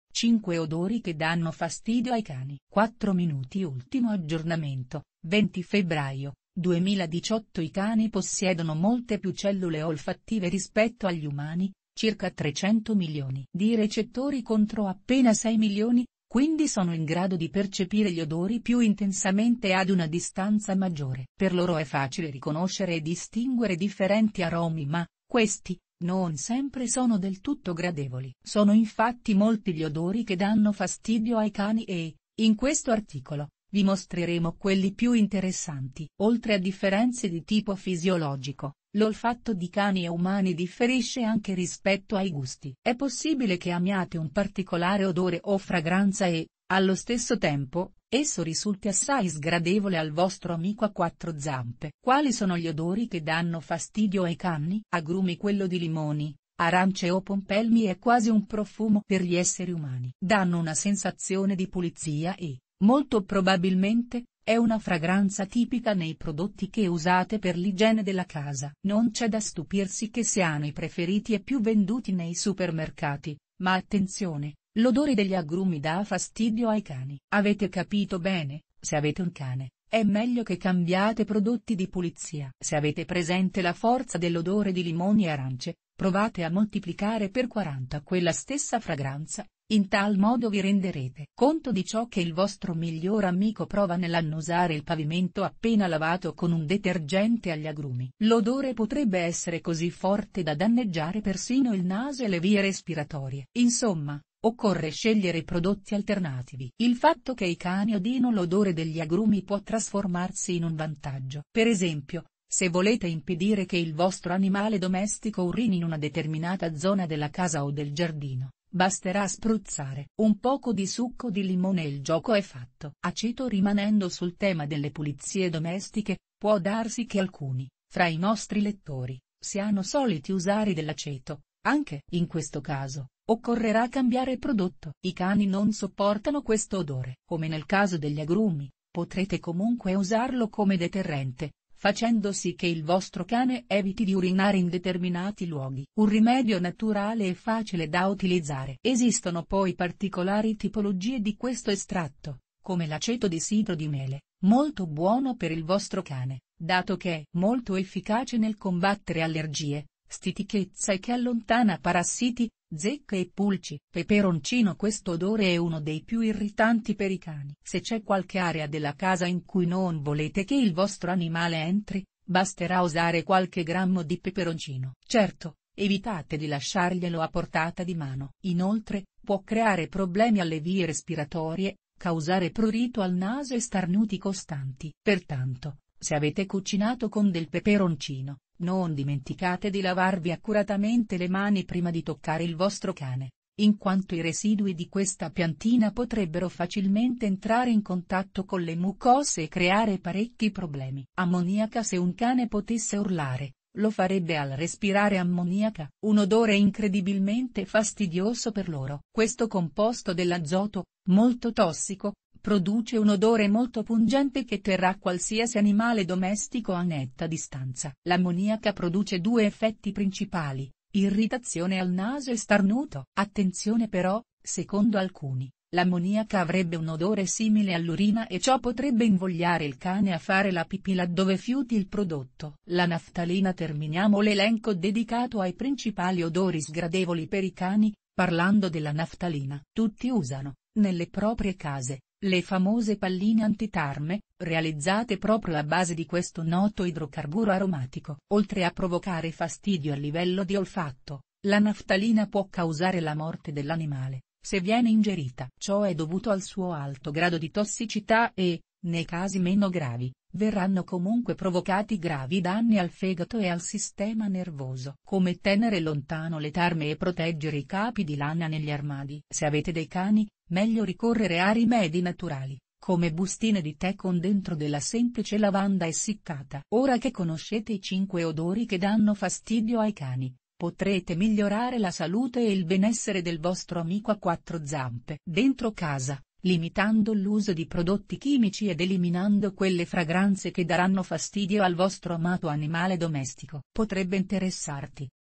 5 odori sgradevoli che danno fastidio ai cani - I miei animali
5 odori che danno fastidio ai cani

I cani possiedono molte più cellule olfattive rispetto agli umani (circa 300 milioni di recettori contro appena 6 milioni), quindi...